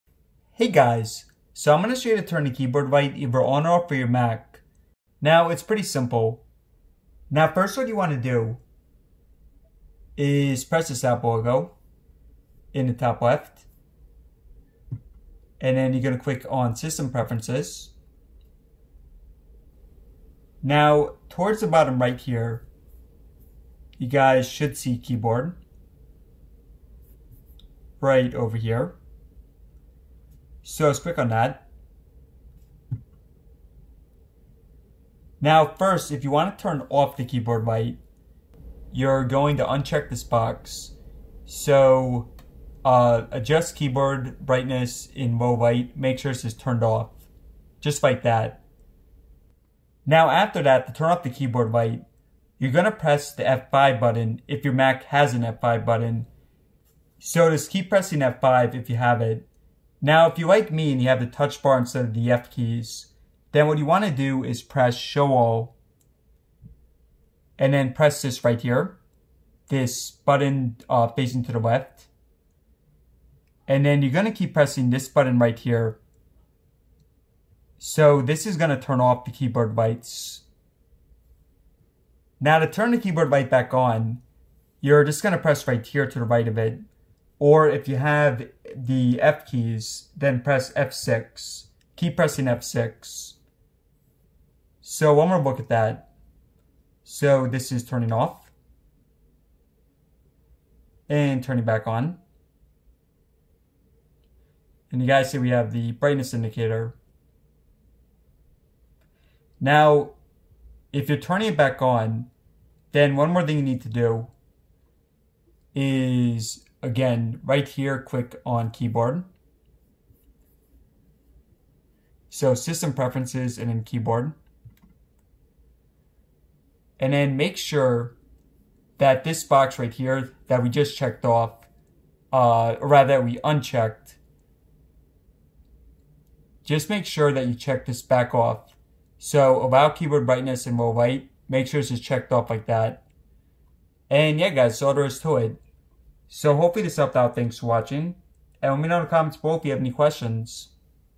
How to Turn Keyboard Light ON or OFF & Adjust Brightness MacBook Pro 16
How to turn your keyboard light on or off and also change brightness on it. MacBook Pro 16 used here, but works for other Macs as well.